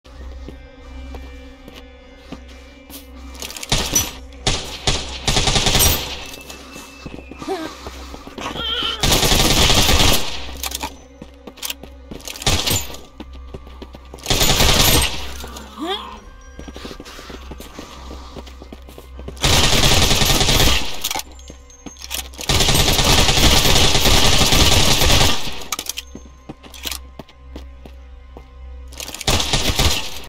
Just A "Normal" Zombie Game
This is a free game on Steam. It has no other objective but to kill zombies.